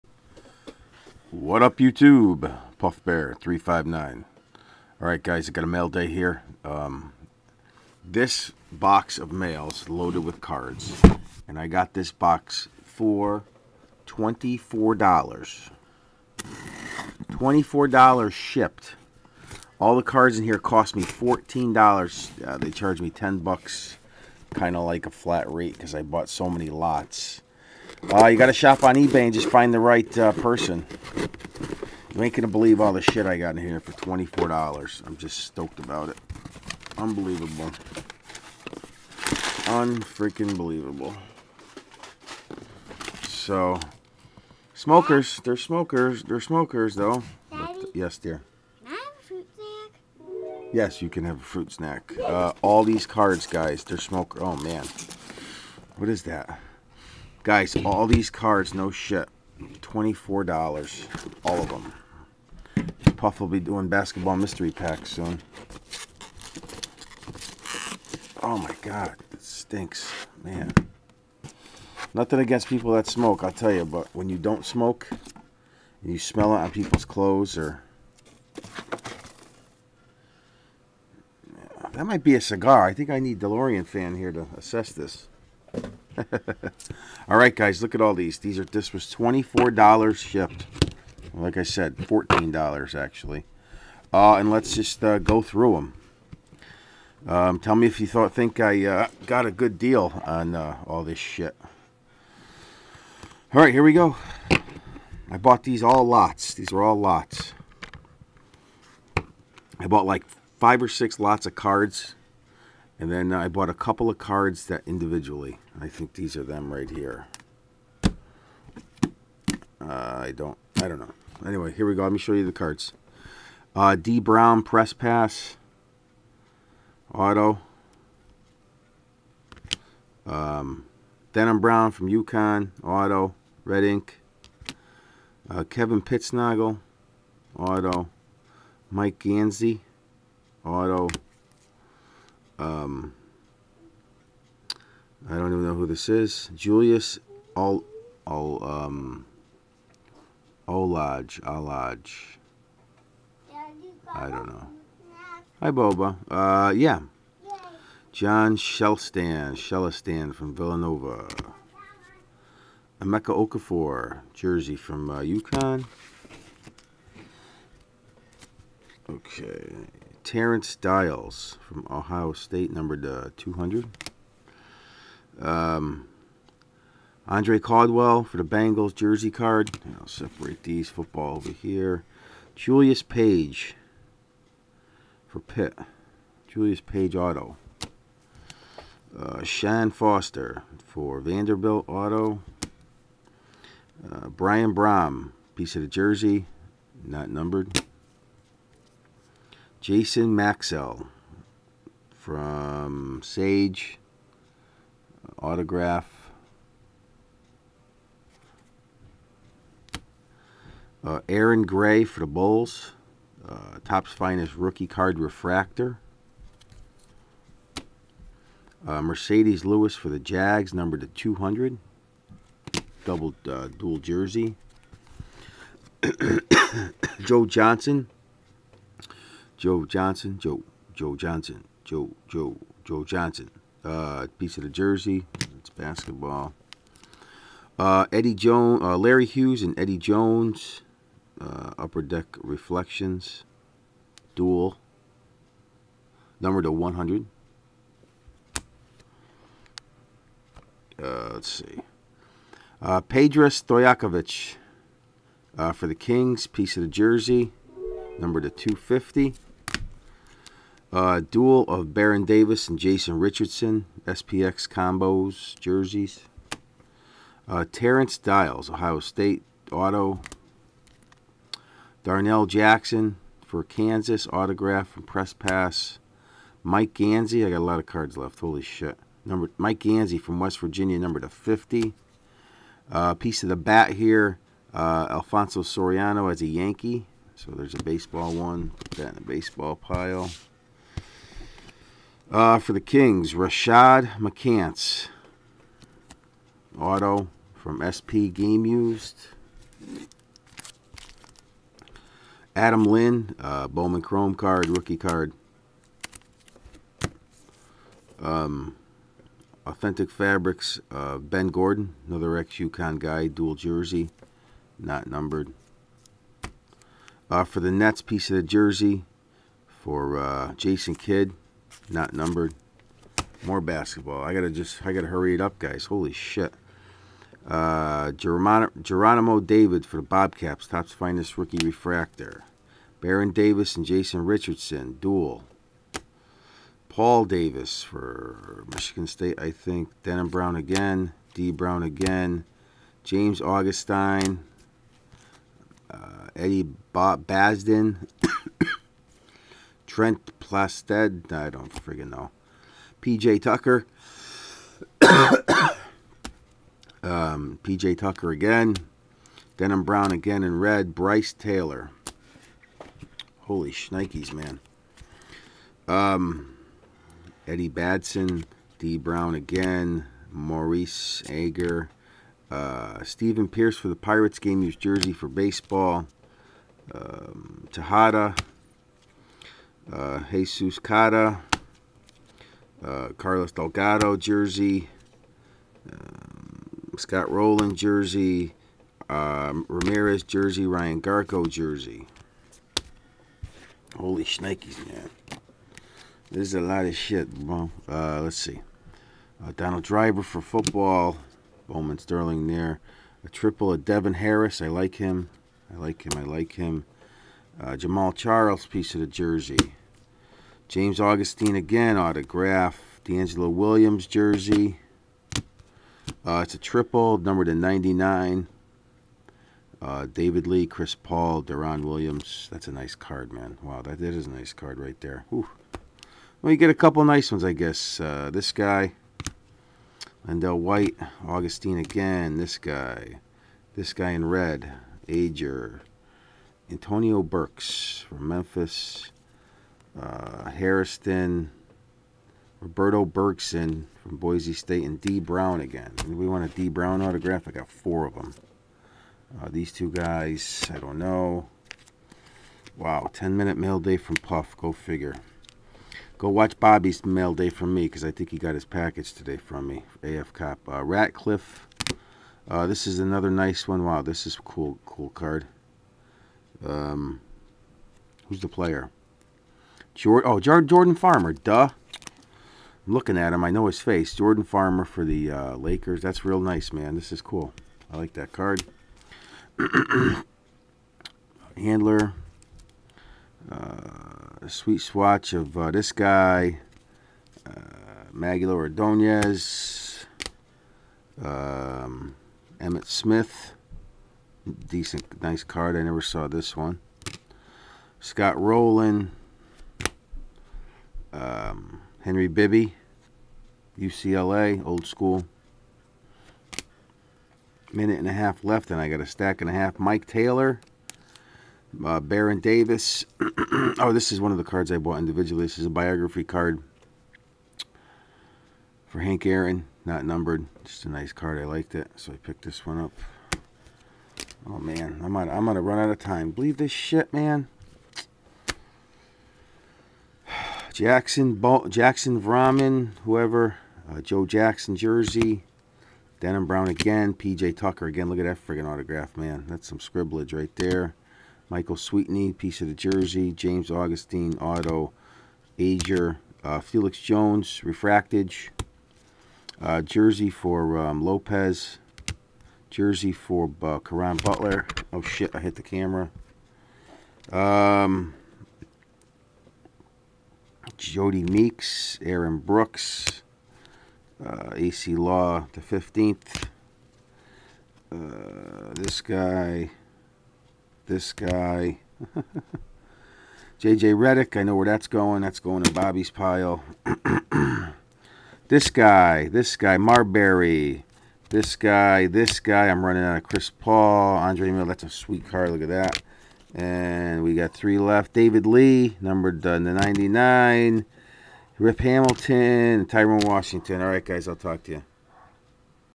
MONSTER Auto & Game-Used LOTS from eBay!!
I bought all these cards for $24.00 SHIPPED!  I thought it was a good deal, let me know what you think.  ENJOY